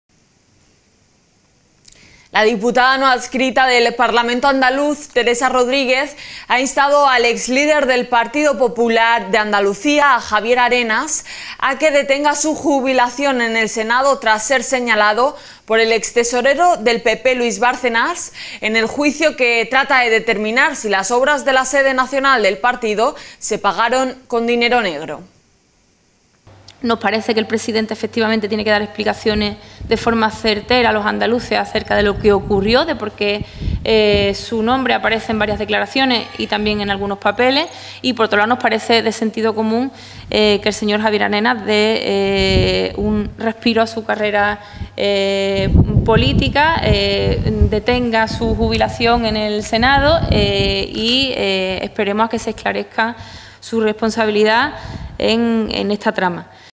🔴Noticia - Teresa Rodríguez insta a Moreno a que explique su aparición en los papeles de Bárcenas
La diputada no adscrita del Parlamento andaluz Teresa Rodríguez ha pedido al presidente de la Junta, Juanma Moreno, que explique por qué su nombre aparece en los papeles de Bárcenas y a Javier Arenas que “detenga su jubilación en el Senado”.

English:

The non-attached deputy of the Andalusian Parliament Teresa Rodríguez has asked the president of the Board, Juanma Moreno, to explain why his name appears on the papers of Bárcenas and Javier Arenas to "stop his retirement in the Senate."

Live streaming